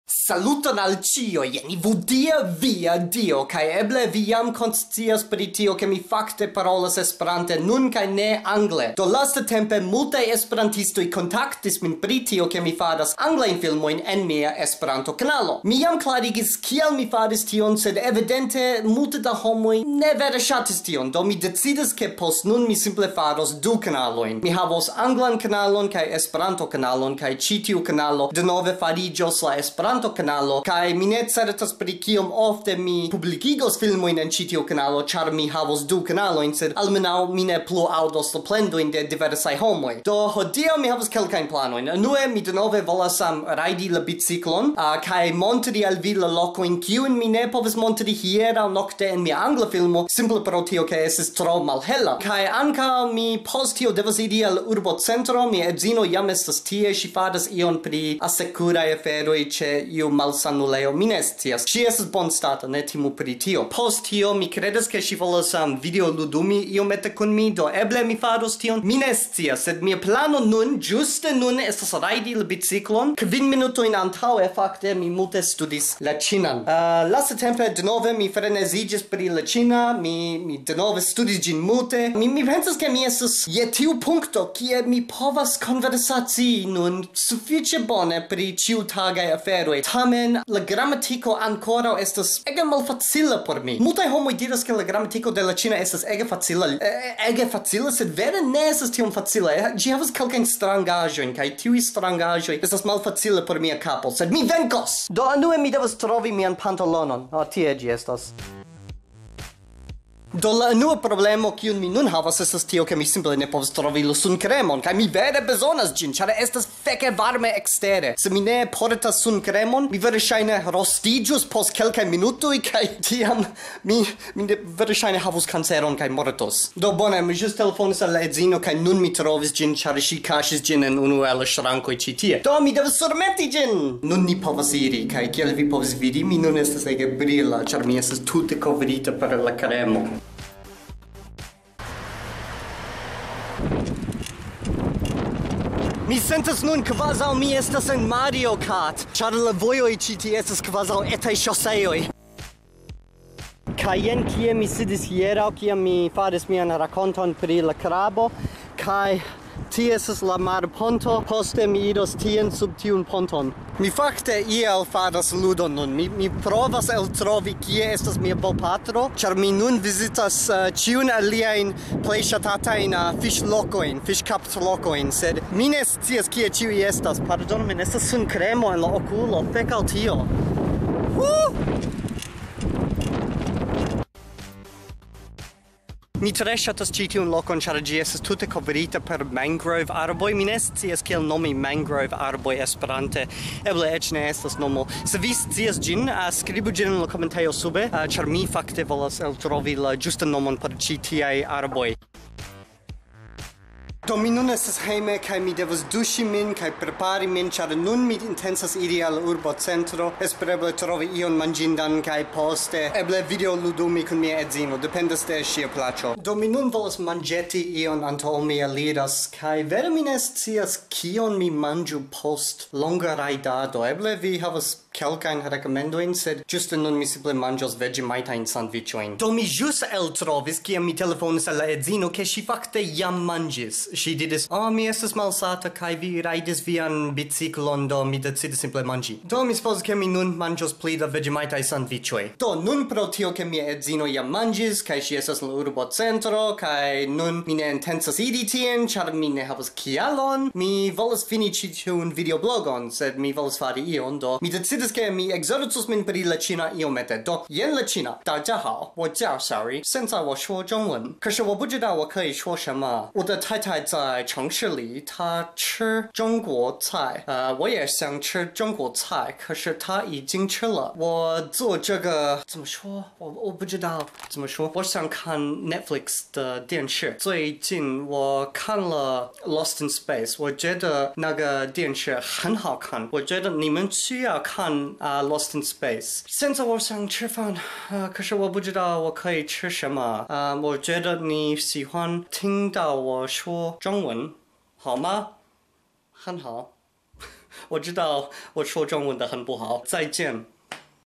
Esplorado kaj ĉina ekzercado
En ĉi tiu videoblogo, mi esploras mian najbarejon kaj ekzercas min per la ĉina.